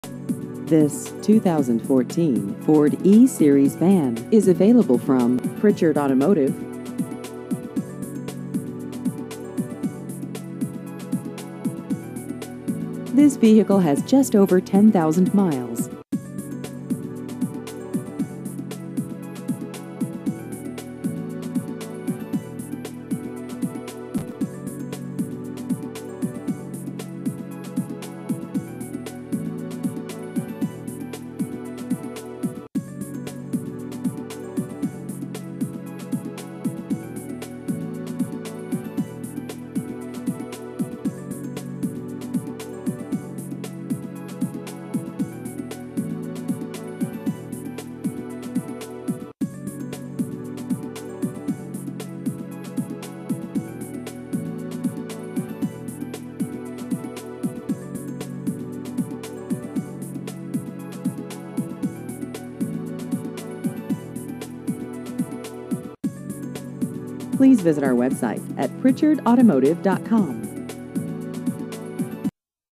2014 Ford E-Series Van Used Cars Ithaca NY
This 2014 Ford E-Series Van is available from Pritchard Automotive.